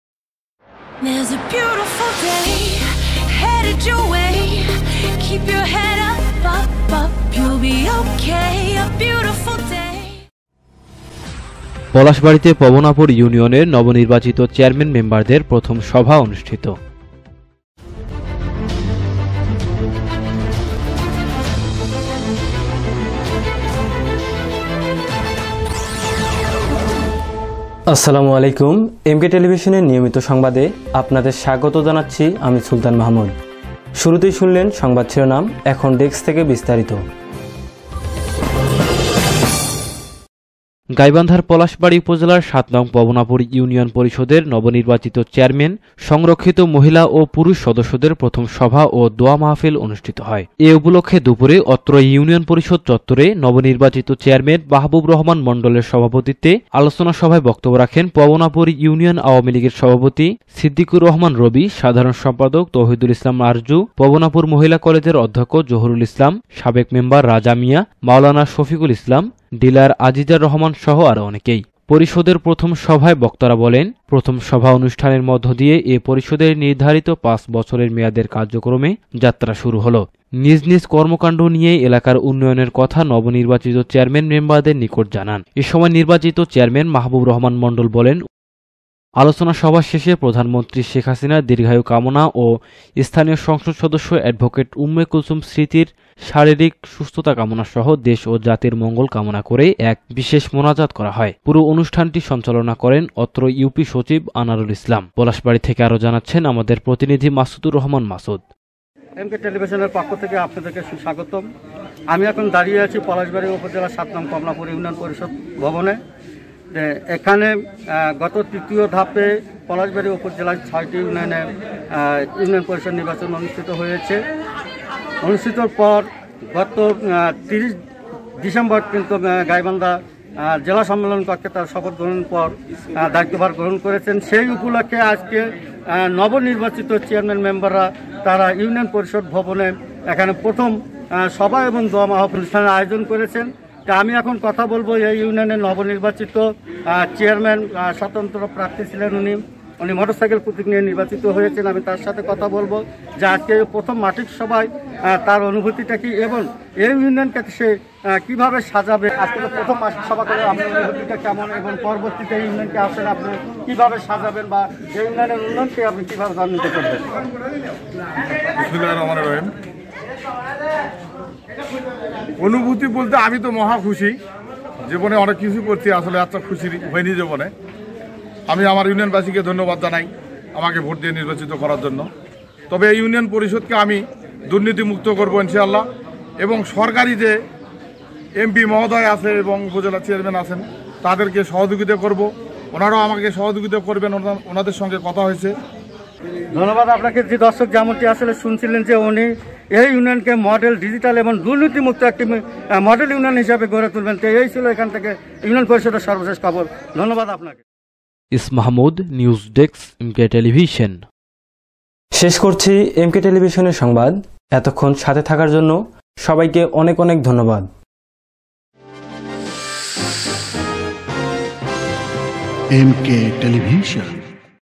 পলাশবাড়ীতে পবনাপুর ইউনিয়নের নব—নির্বাচিত চেয়ারম্যান মেম্বারদের প্রথম সভা ও দোয়া অনুষ্ঠিত।MK Tv
পলাশবাড়ীতে পবনাপুর ইউনিয়নের নব—নির্বাচিত চেয়ারম্যান মেম্বারদের প্রথম সভা ও দোয়া অনুষ্ঠিত